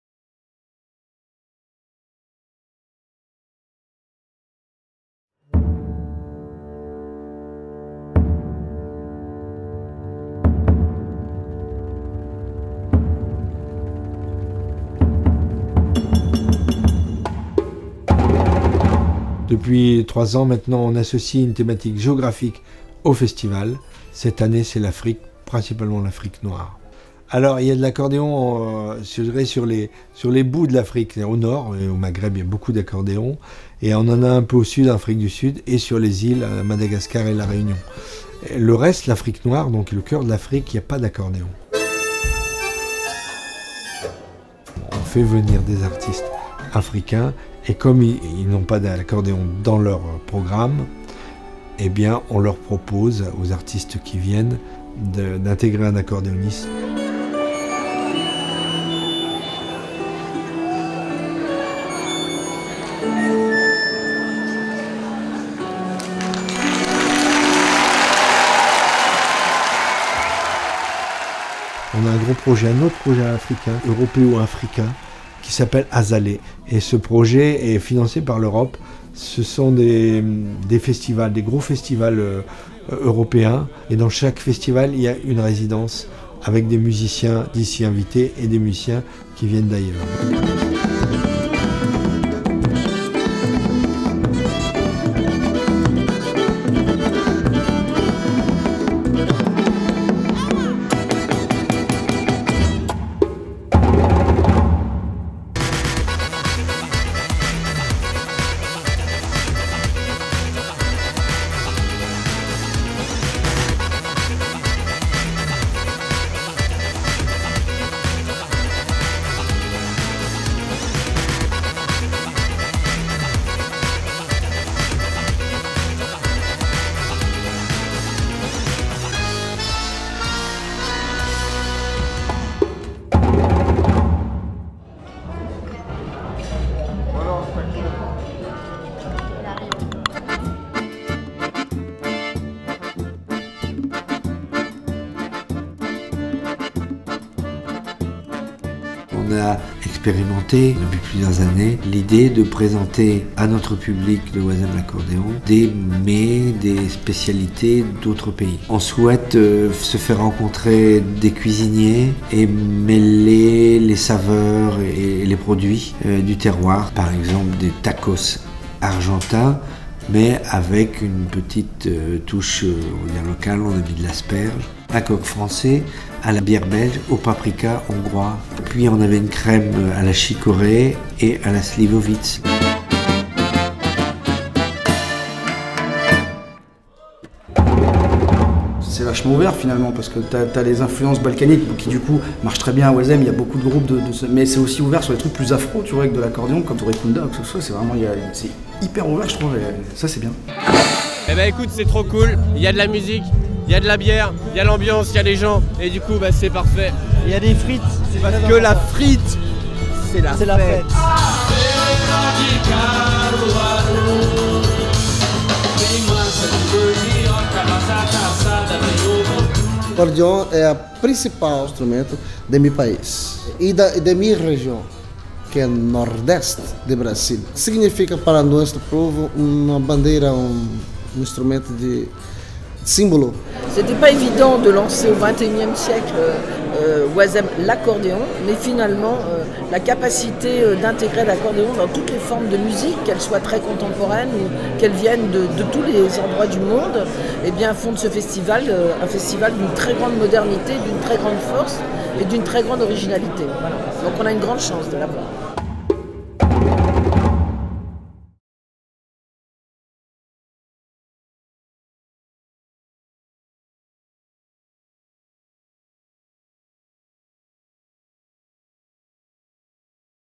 Retour sur Wazlac 2013 ! by Alex Rabozzi
Réalisation : Alex Rabozzi

Le Festival Wazemmes l’Accordéon est un festival populaire de musique. Organisé par l’asso Flonflons, ce festival est se déroule dans le nord de la France, dans un quartier de Lille, Wazemmes. Plusieurs artistes de renom, une atmosphère débordante d’énergie et beaucoup, beaucoup de bonne humeur ! Pétanque, frites, vélo, concerts, bals,  … Rien de tel pour s’éclater un max et fêter l’arrivée de l’été !

Flonflons est une association culturelle lilloise, organisatrice d'événements et productrice de spectacles et de CD.